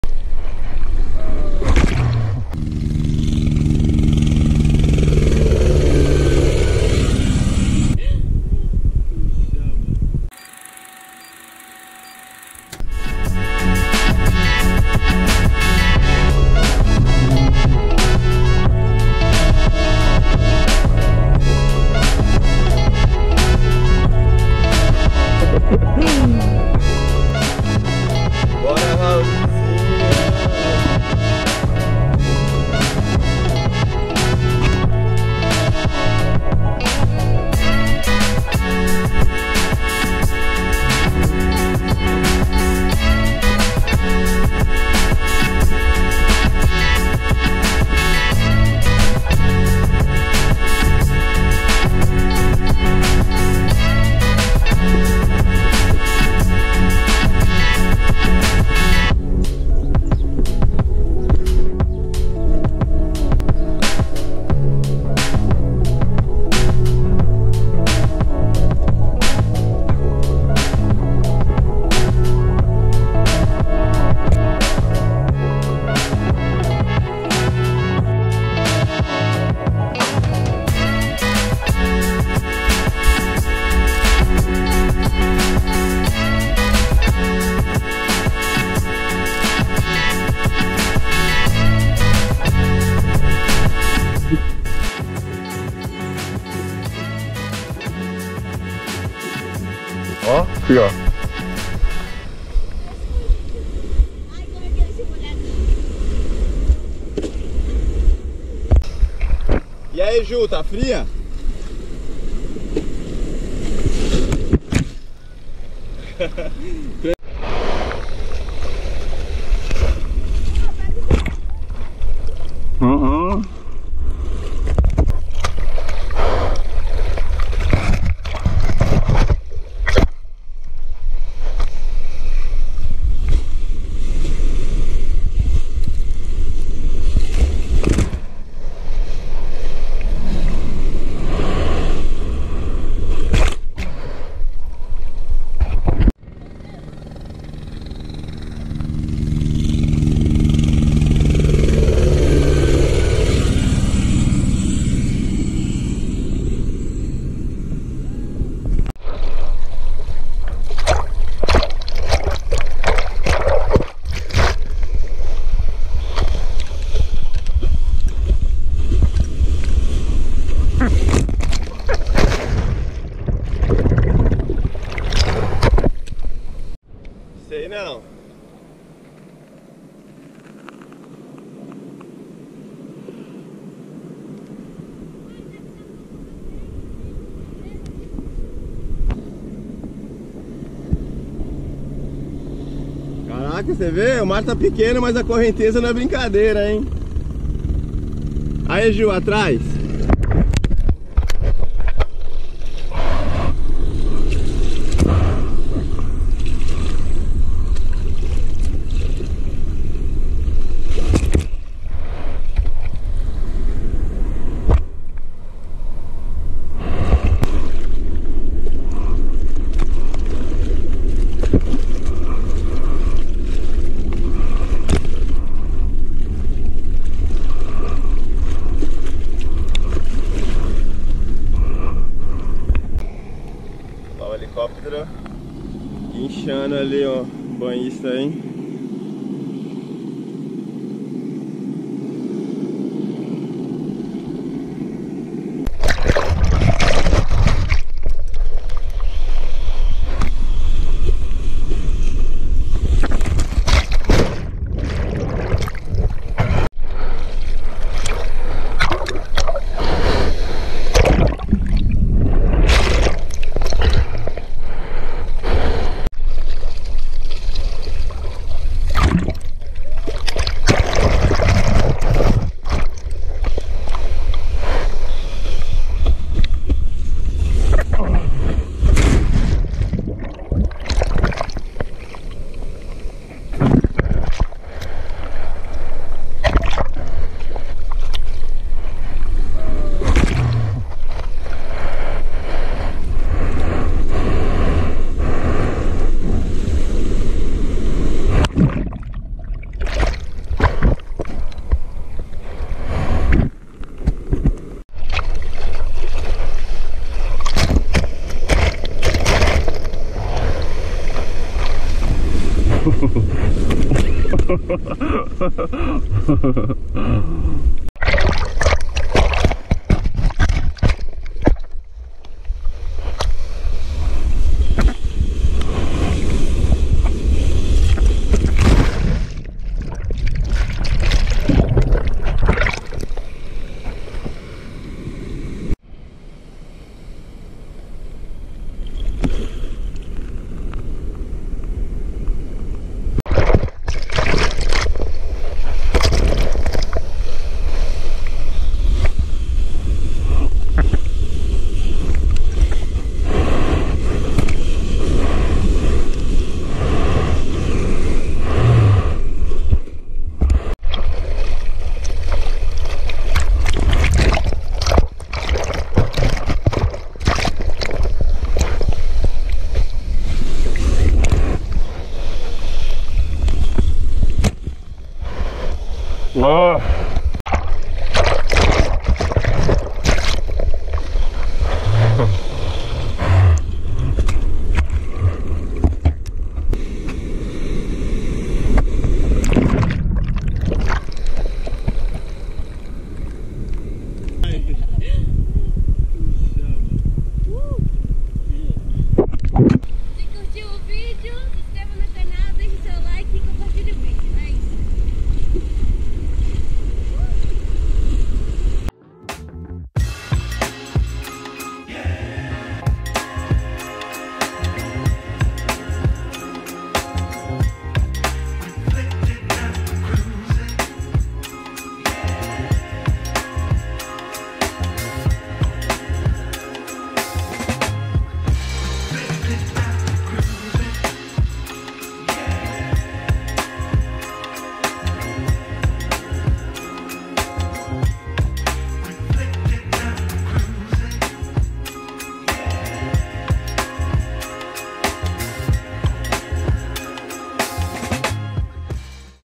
FREESURF GOPRO NATAL E MAROLINHAS JUQUEHY SURF
Surf e skate um estilo de vida Segue a vivencia Aventura em familia!!
Natal rendeu um surf no canto. Mar pequeno mas sempre rende a diversão!!
um pouco de correnteza e banhistas foram puchado, o elicoptero veio ao resgate e deu tudo certo.
Seguimos firme em busca da evolução, sempre se divertindo e aproveitando ao maximo cada momento. seja no mar ou na pista de skate a vibe em familia não muda é sempre muita vivencia!!
Praia de Juquehy litoral norte de São paulo cidade São sebastião é onde moramos e aproveitamos a vida desta maneira, com muito esporte e vontade de aprender cada dia mais.
Se inscreve para nos acompanhar e ver mais conteudo ❤👨‍👧‍👦

Abraços  Julia & Miguel ❤